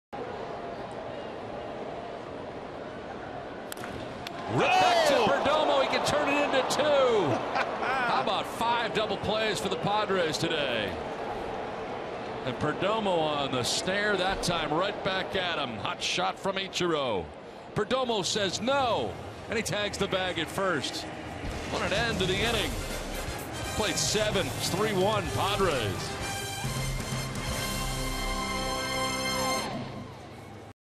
SD@MIA: Perdomo completes an unassisted double play
Luis Perdomo snares Ichiro Suzuki's comebacker to the mound and then comfortably throws to first for an unassisted inning-ending double play